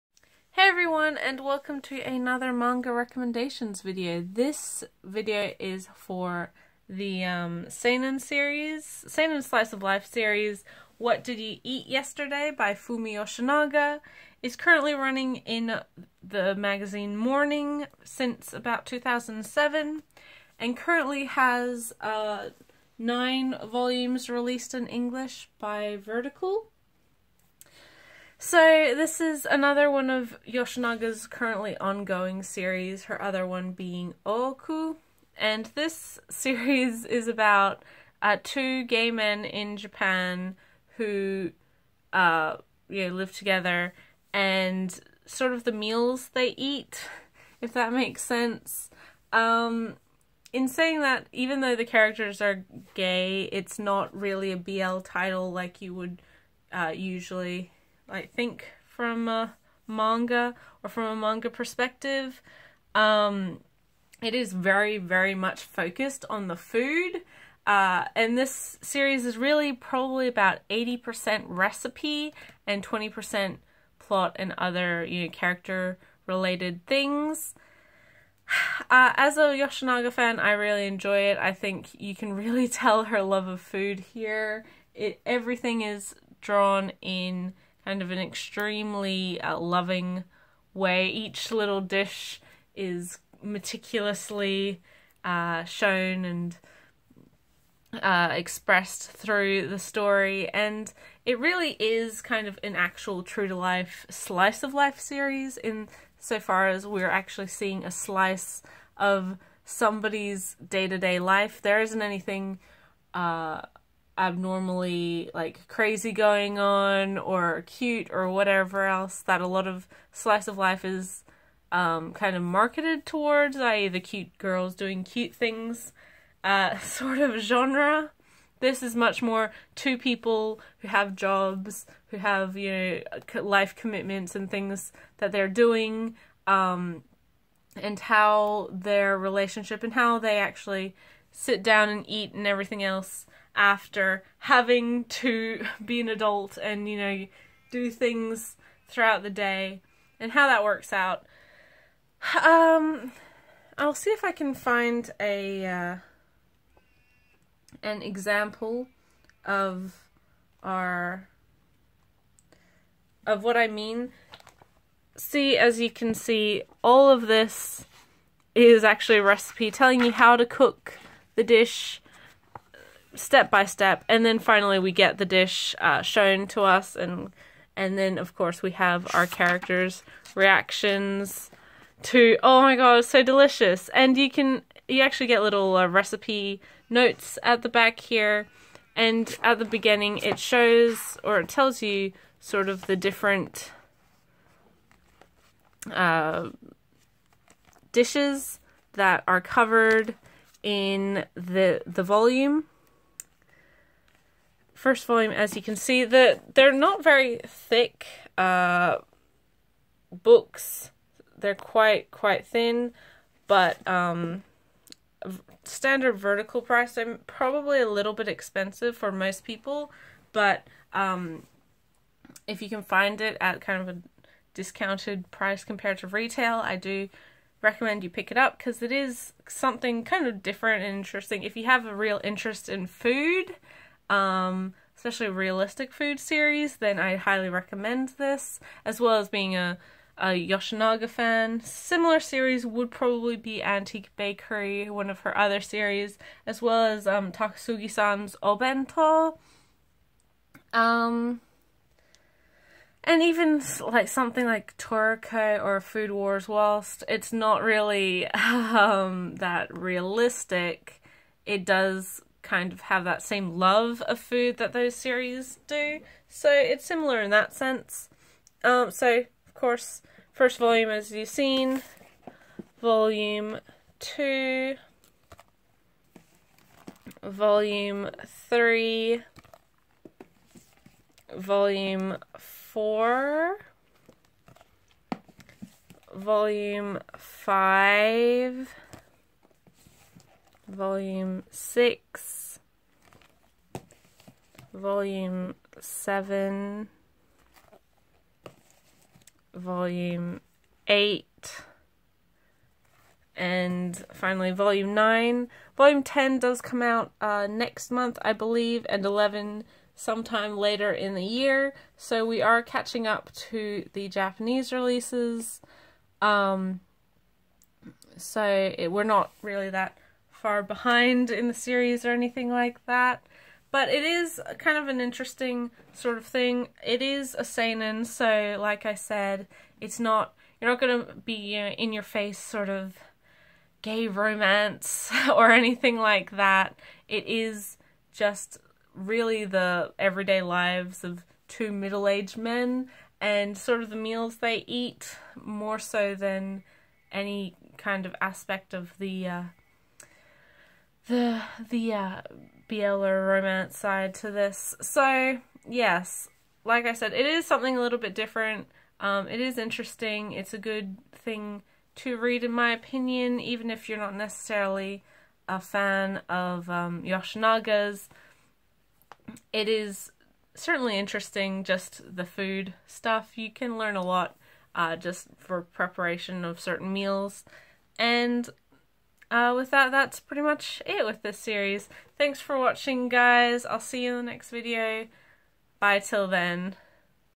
Manga Recs || What Did You Eat Yesterday?
Fumi Yoshinaga's seinen slice of life story that is more recipe than plot!